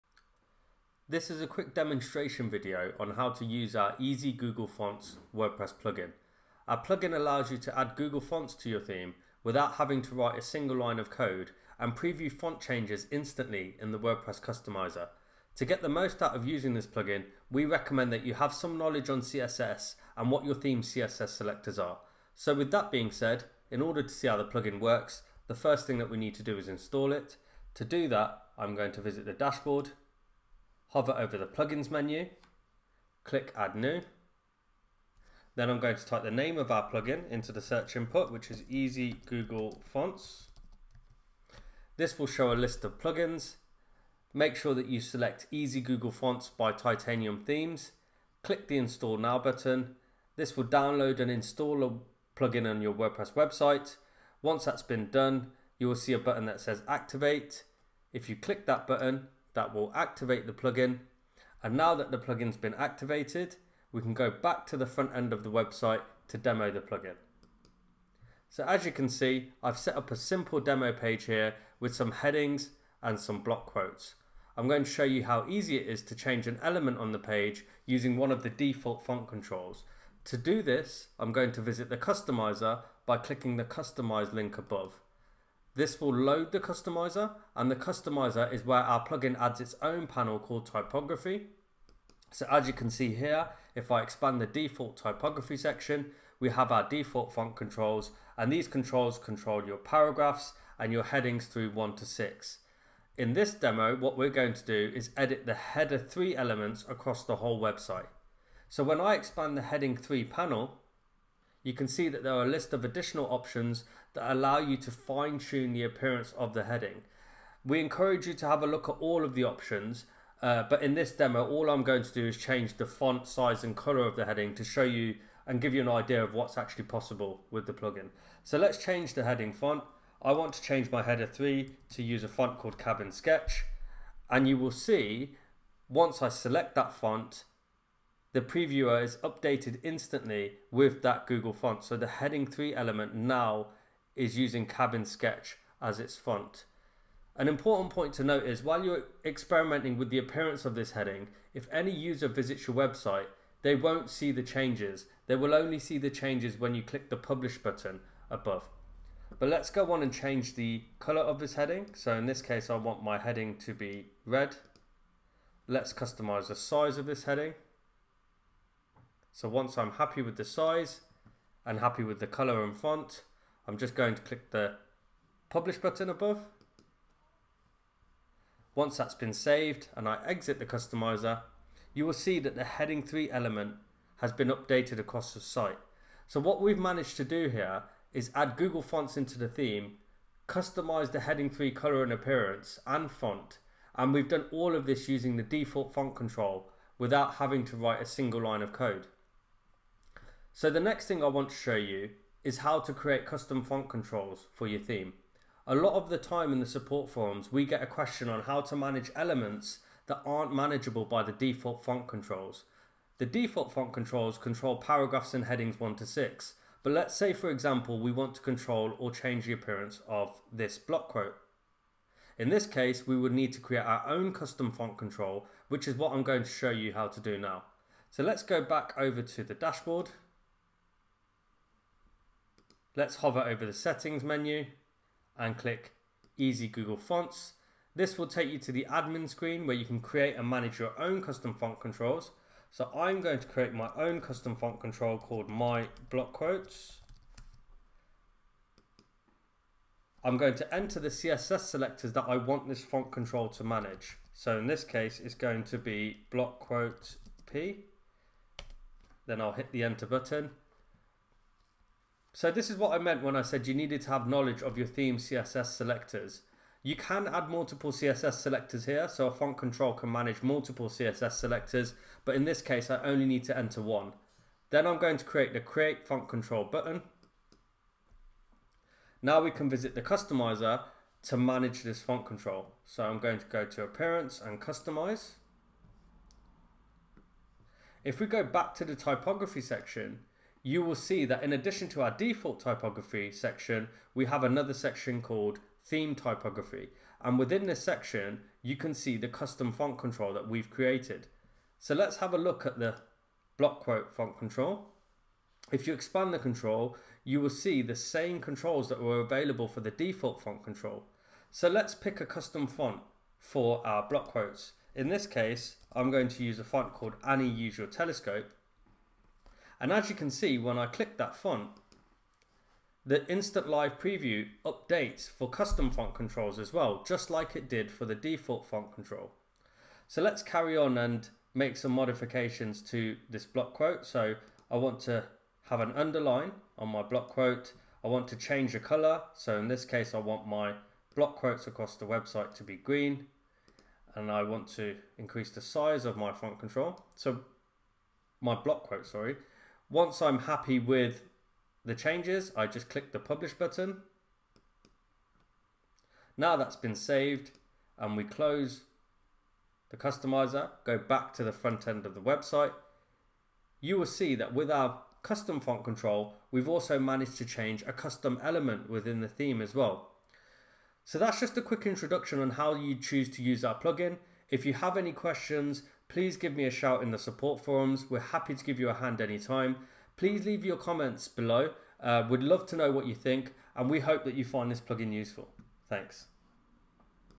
Easy Google Fonts: A quick and easy way to add google fonts to your WordPress Theme
This is a tutorial video for our WordPress google fonts plugin called Easy Google Fonts.

We're building something exciting.

A beautifully designed WordPress theme with a first class Gutenberg editing experience. Sign up to be notified when it's ready

00:00 Introduction
00:22 Plugin Installation and activation
01:08 Default font controls customizer demo
03:19 How to create your own custom font controls
04:38 Custom font control customizer demo
06:10 Outro

Plugin Summary

This plugin allows you to take full control of your theme's typography in any WordPress theme (no coding required). It allows you to choose from all available google fonts and font variants to insert into your website without coding.
It also allows you to preview font changes on your website in realtime using the WordPress customizer, allowing you to preview what your theme will look like with the new google fonts before you save any changes.